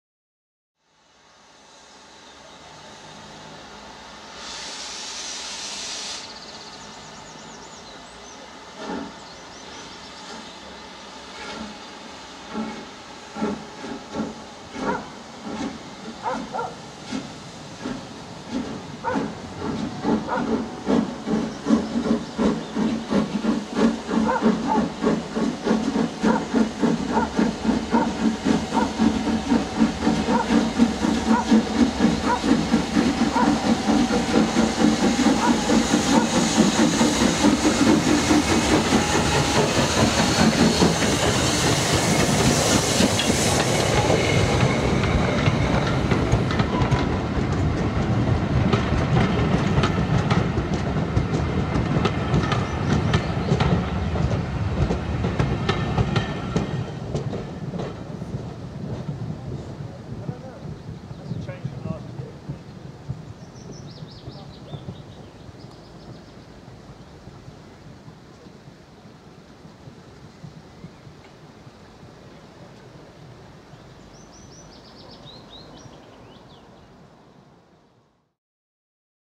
Lambton 29 & 45407: Goathland: 27/04/2014
The North Yorkshire Moors Railway staged their annual Spring Steam Gala over two weekends from 25th-27th April and 2nd-5th May 2014. The first Sunday of the gala sees the unusual pairing of home-based Lambton Tank No. 29 and Ian Riley's 'Black Five' No. 45407 as they emerge from a heavy fog at Goathland hauling the 09.28 from Grosmont to Pickering.